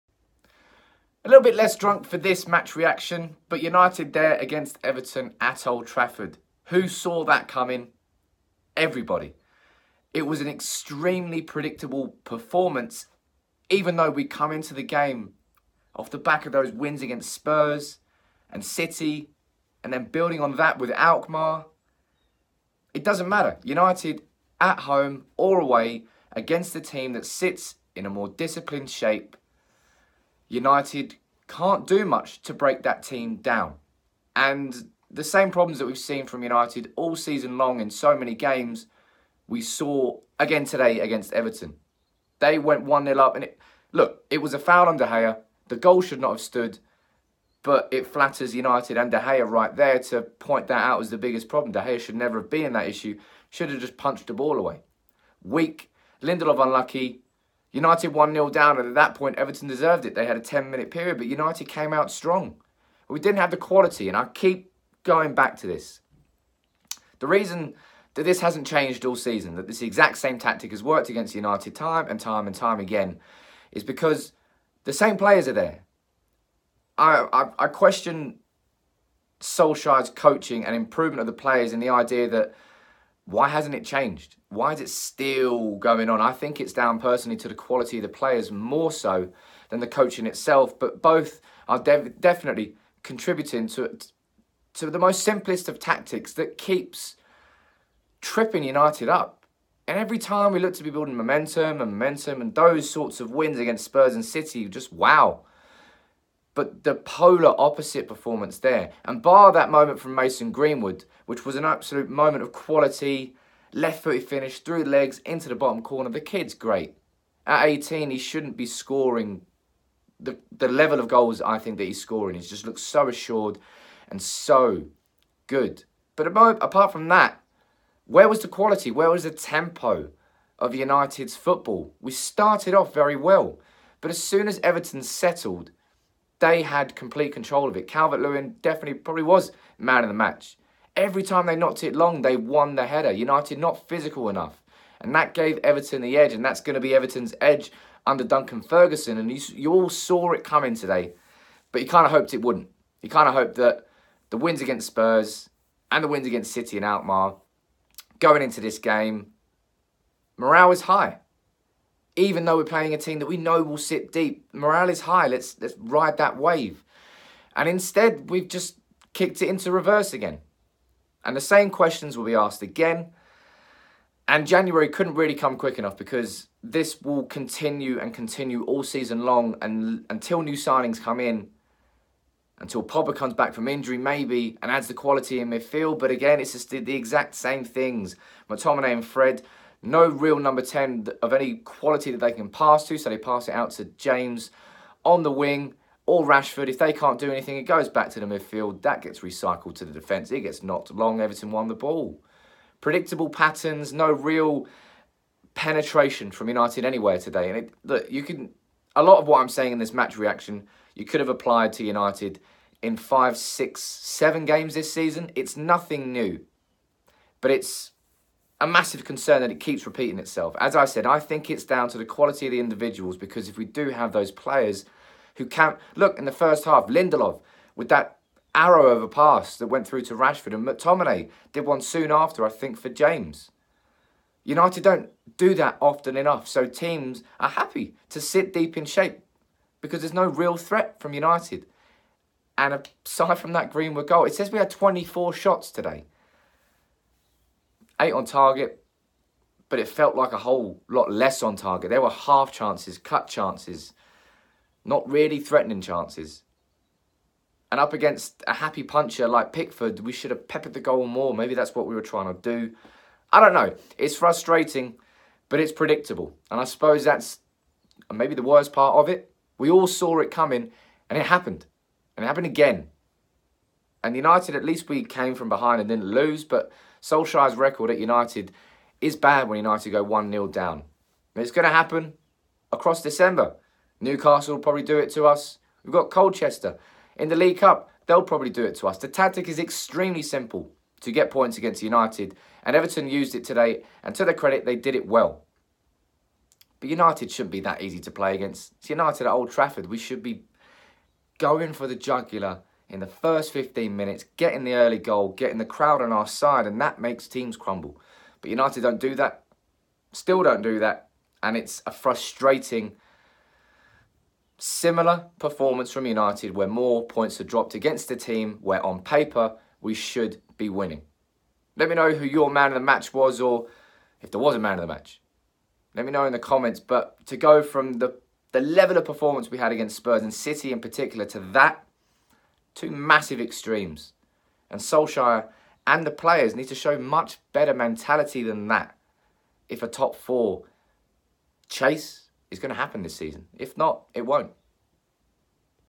SAME PREDICTABLE PROBLEMS | MAN UTD 1-1 EVERTON
Man United's 1-1 draw with Everton highlighted the same problems Ole Gunnar Solskjaer's Man Utd team have had all season. It was predictable, poor and painful to watch from a United team who headed in to the game with fantastic form after beating Spurs, City and AZ Alkmaar. Instead of building on it, United have taken a step backwards and the same questions will be asked about United and Solskjaer